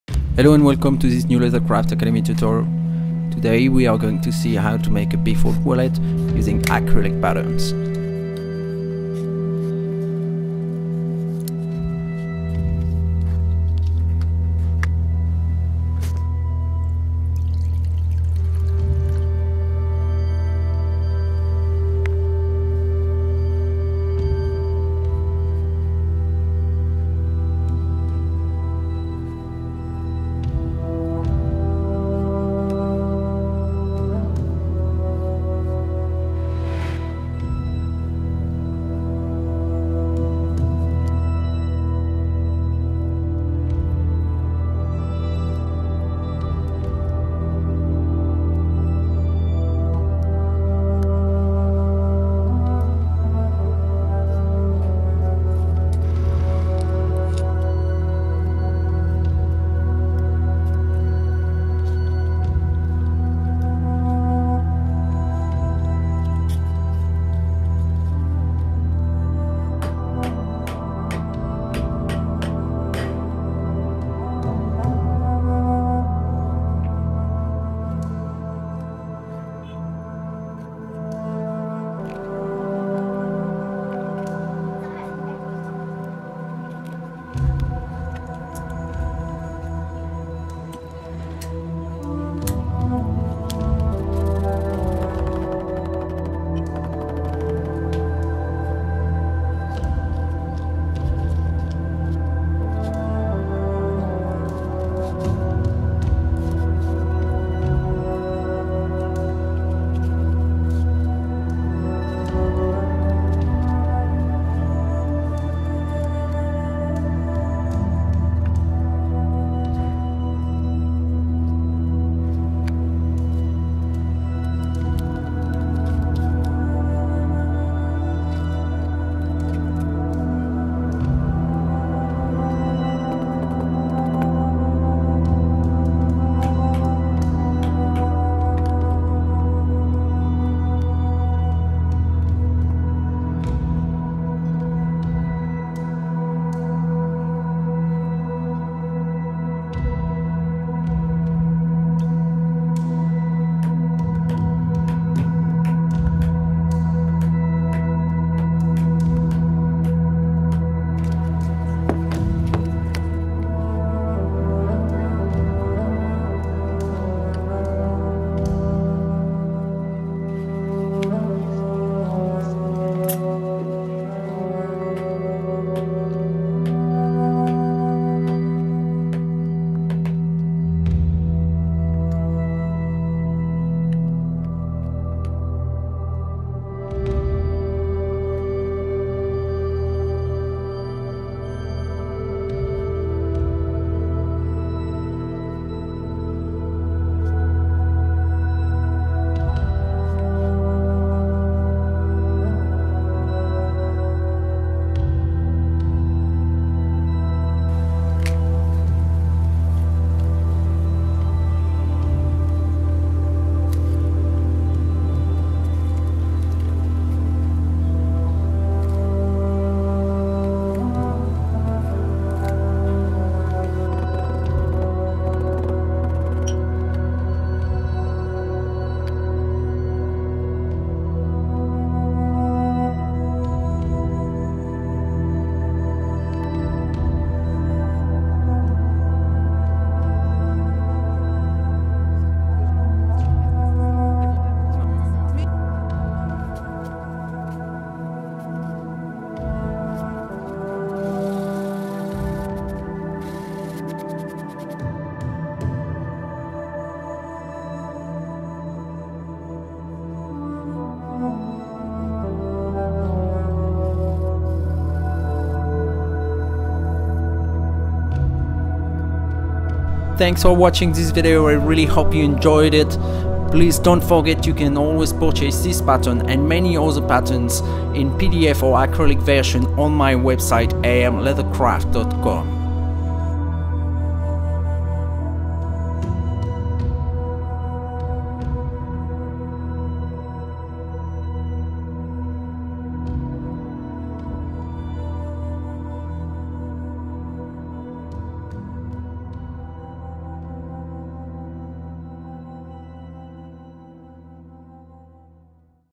Making a simple leather wallet by hand/using acrylic patterns
In this video I am showing you how to make a simple bifold wallet using acrylic templates

List of tools used in this video:
Affordable leather crafting tool kit
List of tools :

- Scratch awl
- NT cutter
- Saddler leather hammer
- Crepe eraser to clean products
- Regad electric creasing equipement